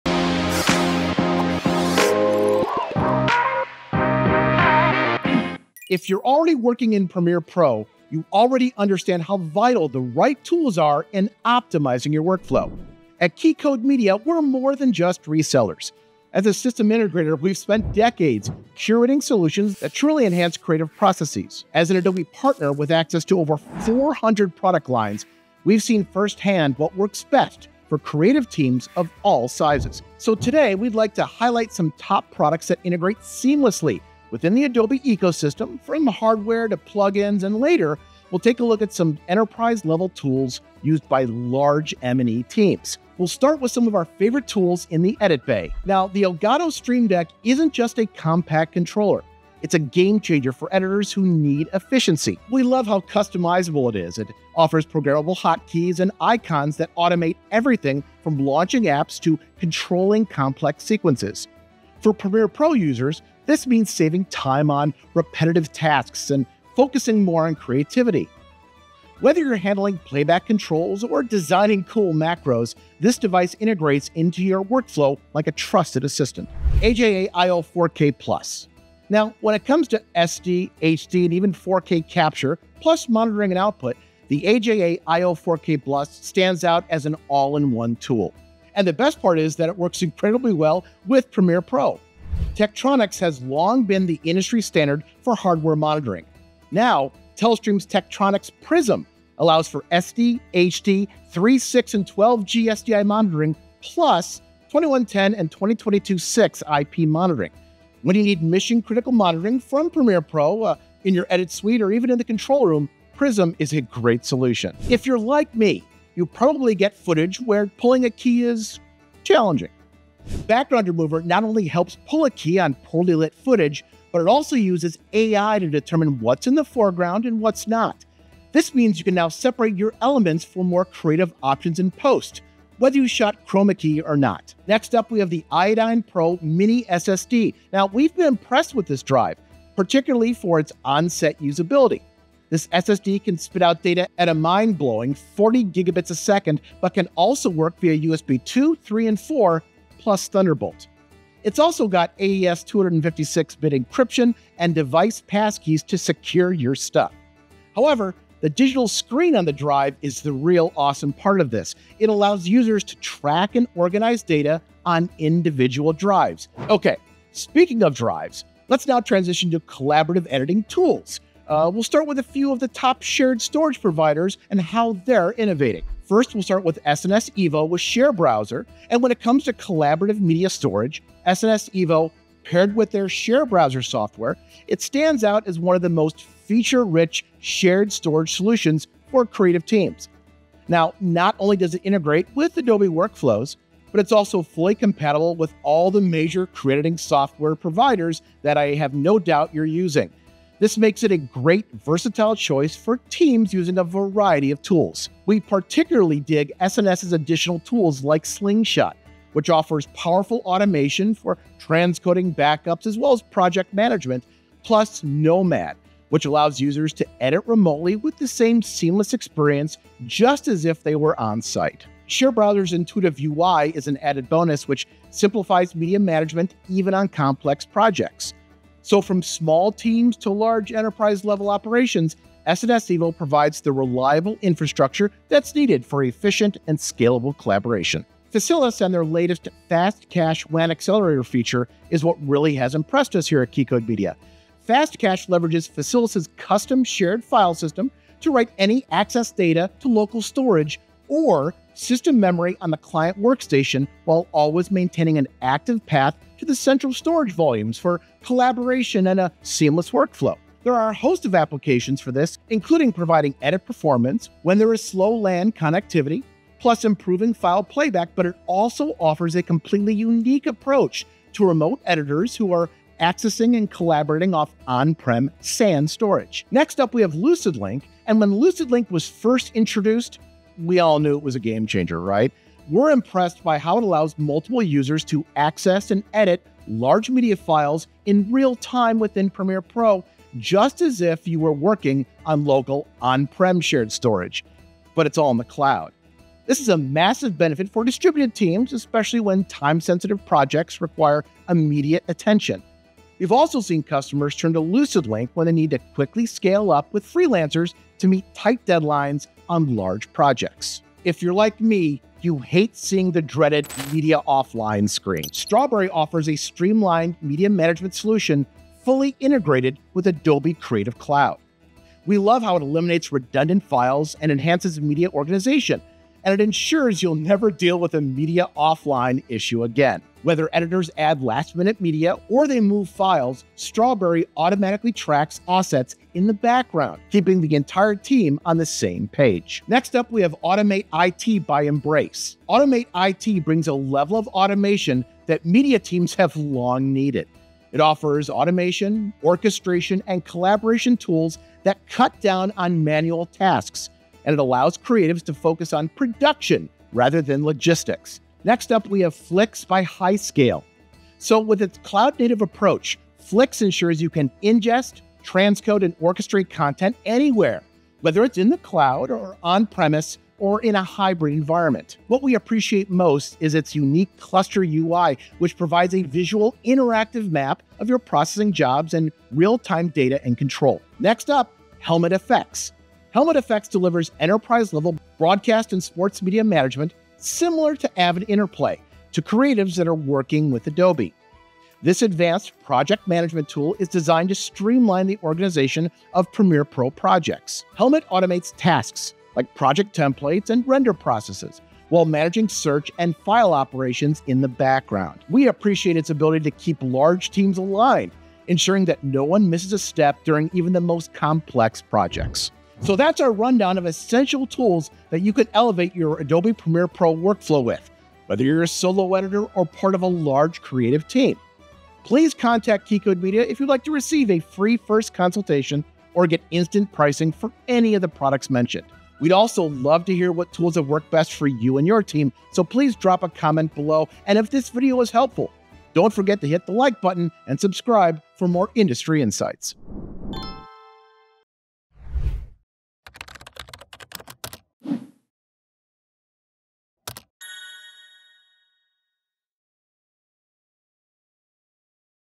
Key Tools for Adobe Premiere Pro Creative Teams
Ever wonder how you can improve your adobe premiere pro workflow? Well no worries we got you covered. Join us on INPUTS/OUTPUTS with our Senior Director of Innovation Michael Kammes as we go over several enhancements from plugins, hardware, software, etc. that will help large and small creative editing teams.

CHAPTERS
00:44 STREAM DECKS
1:17 AJA IO 4K PLUS
1:35 TELESTREAM PRISM
1:56 BACKGROUND REMOVER FX FACTORY
2:13 IODYNE MINI SSD
2:55 SNS EVO
3:58 FACILIS FAST CACHE
4:37 LUCIDLINK PANEL
5:10 PROJECTIVE STRAWBERRY
5:38 AUTOMATE IT
5:57 FLICS
6:20 MOOVIT HELMUTFX